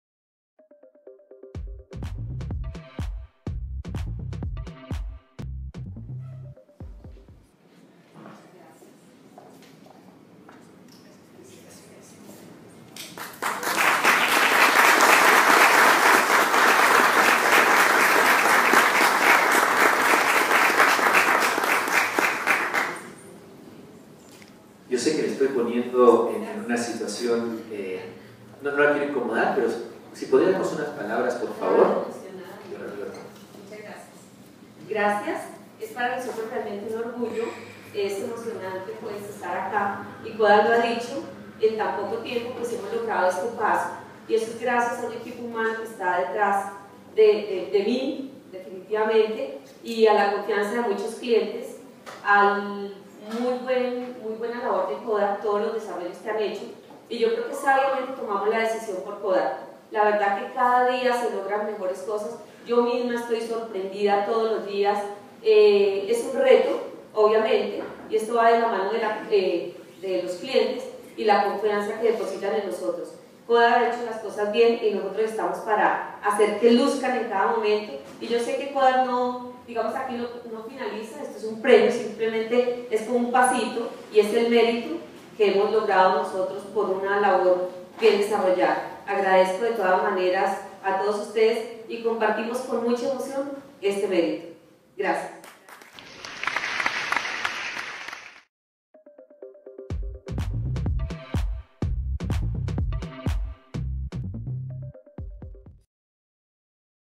Kodak Certificación Flexcel NX
Entrega de la primera certificación Flexcel Plates NX en Colombia, entregada por Luis Medina, gerente de Kodak para América Latina, a la empresa iFlexo.